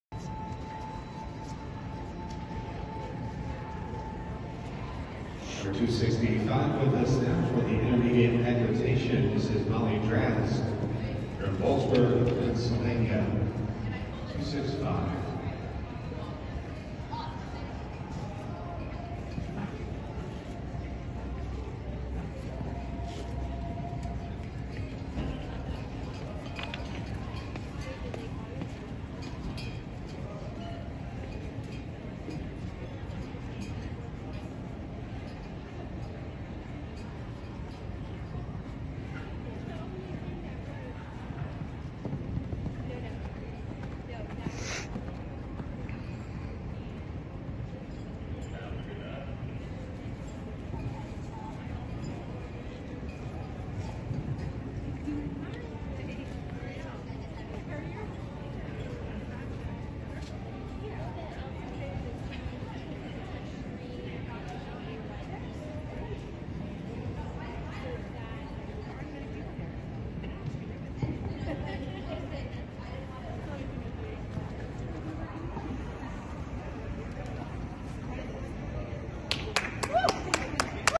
Sidney - Mid Atlantic Equitation Festival
Ridden by Molly Drass to a champion in the Intermediate (3') equitation division. Owned by Carousel Farm in Centre Hall, PA.